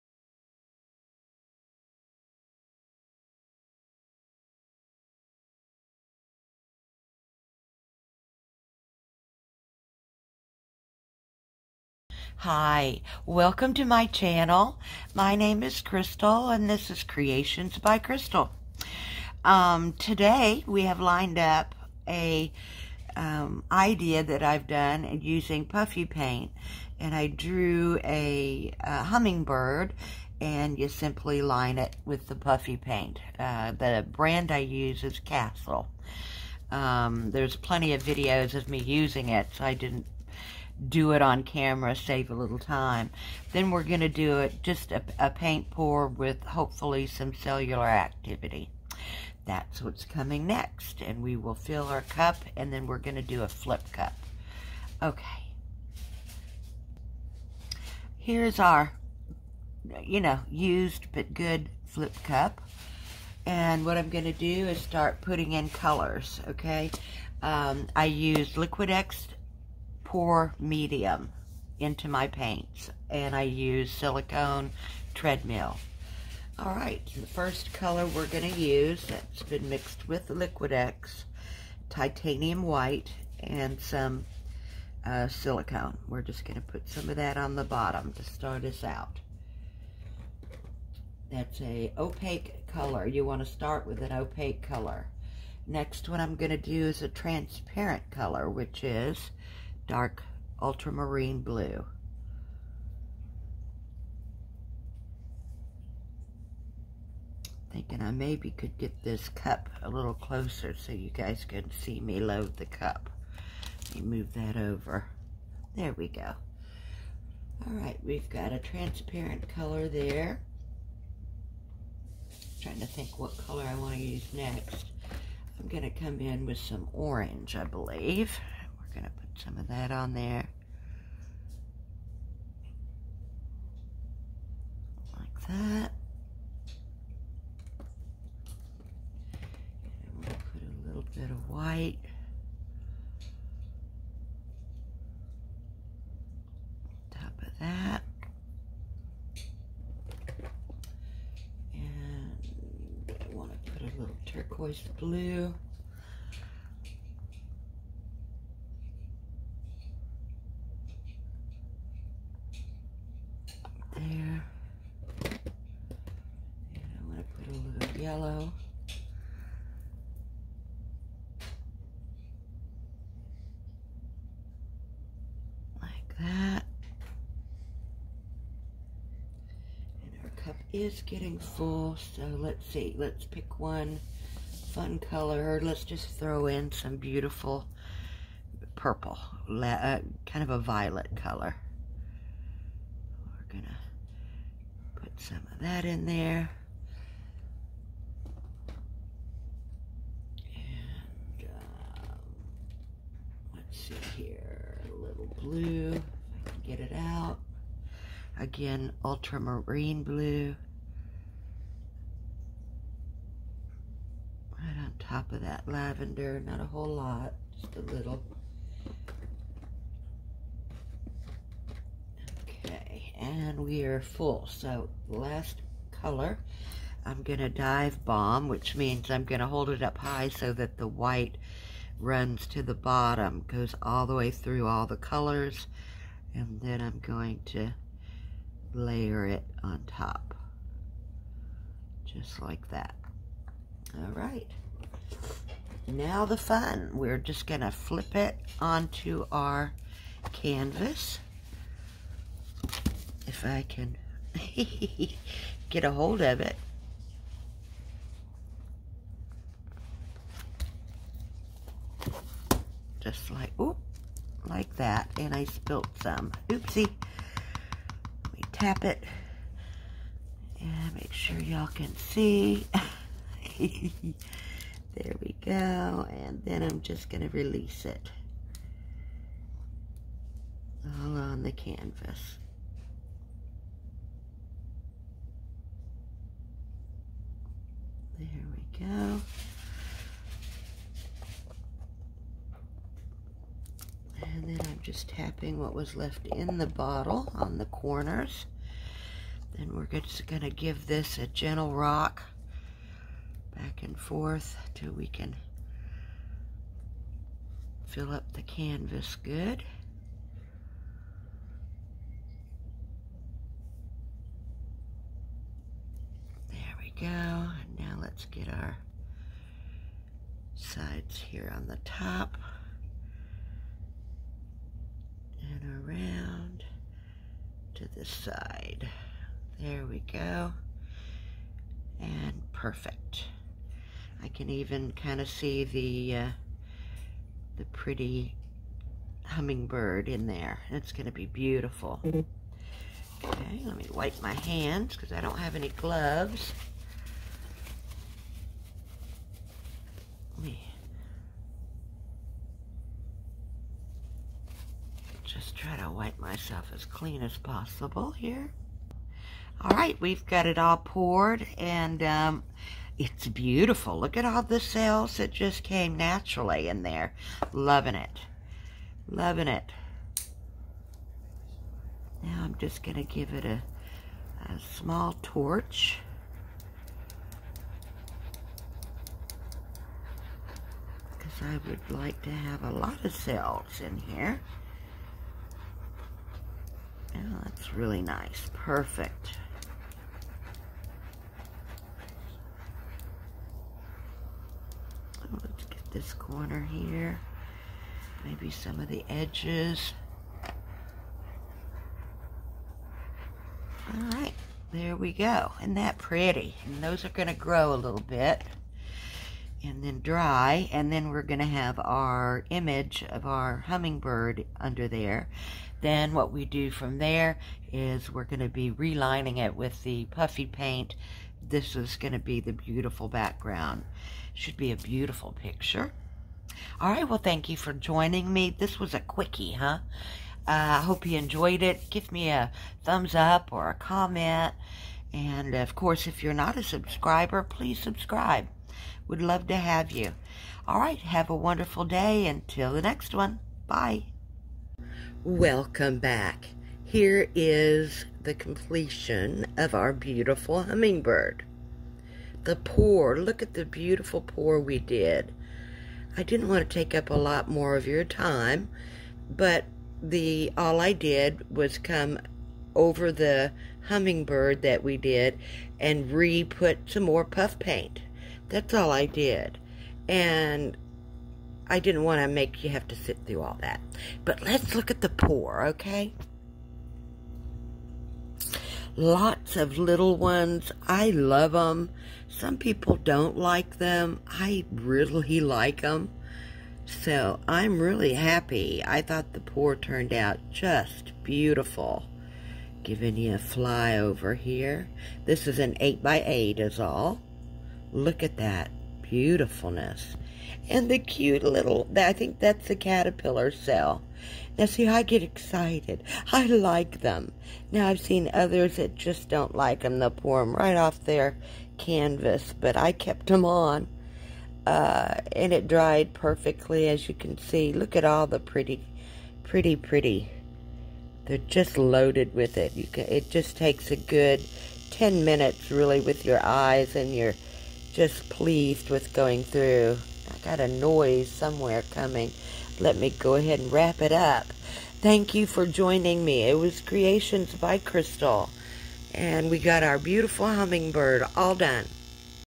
Pour art/ The Hummingbird/ Fluid art/ paintpouring/diy
Hello!  I hope you enjoy my concept of birds in a paint pour AGAIN! LOL This time one of my favorites-The Hummingbird!
I used- Castle PUFFYPAINT
Arteza paints
PM:   is Liquitex pour medium
Silicone: Coconut hair serum, ( walgreens)
inspiration to build on Paintpours goes to
Mii Paintings!  ty Mii